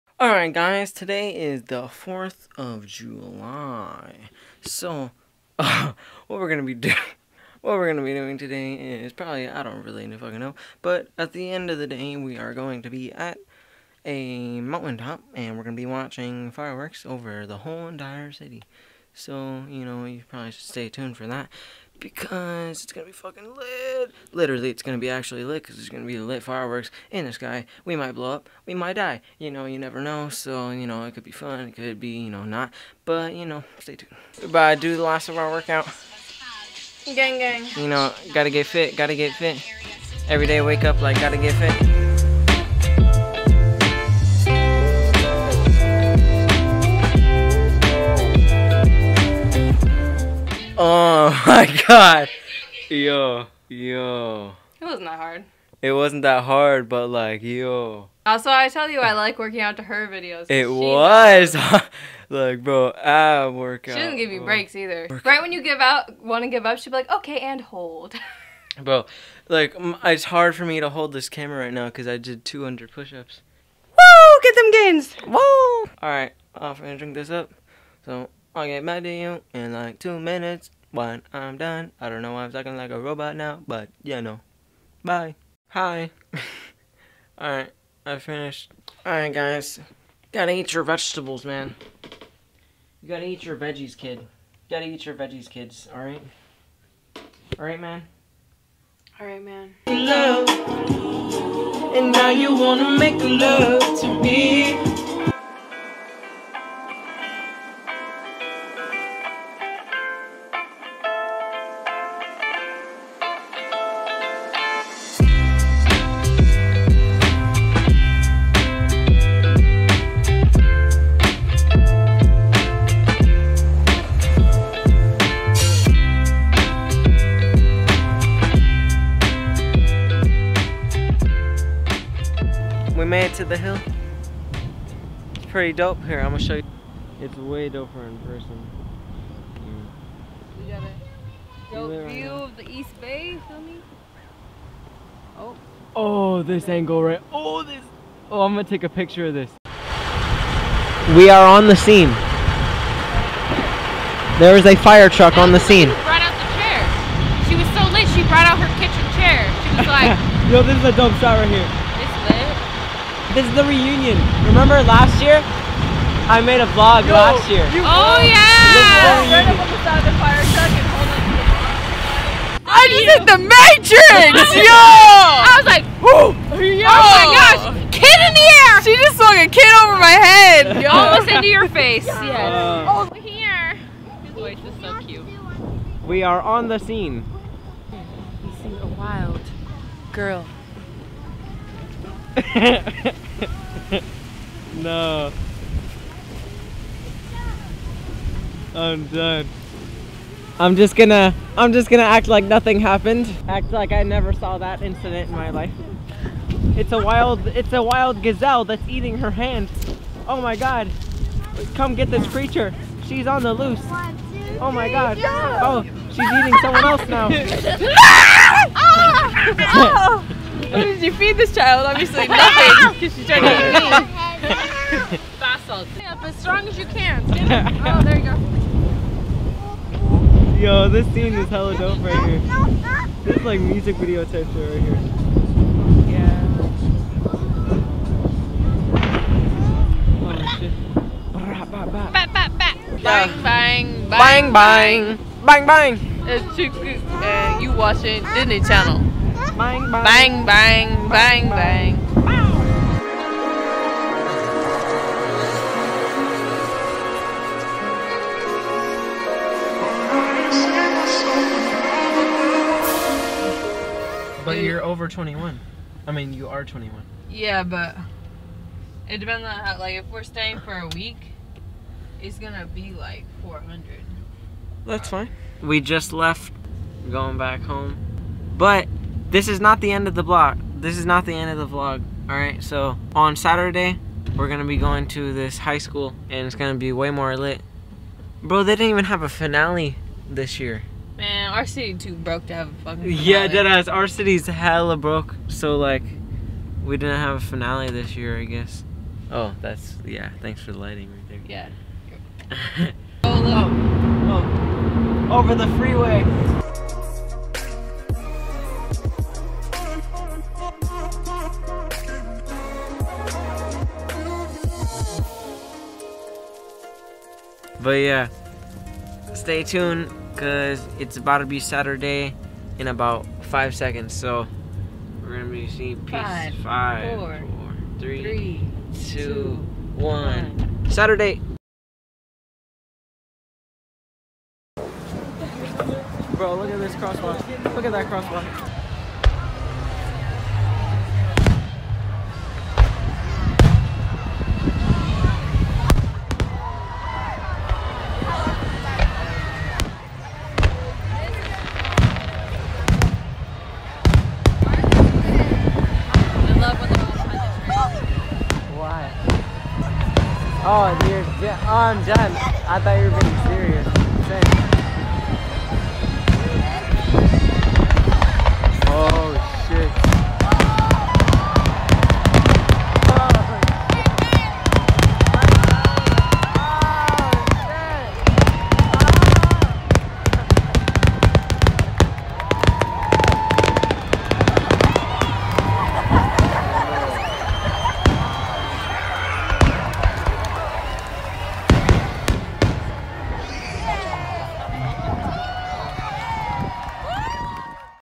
NatalieAndRobert's 4th Of July
HAPPY FOURTH OF JULY, hope you all had a great time out with your friend's or families. If your here from last year's vlog of our 4th Of July then your a real one. Have a great rest of your day :)

Video Edited by - Robert White

Have a great day (: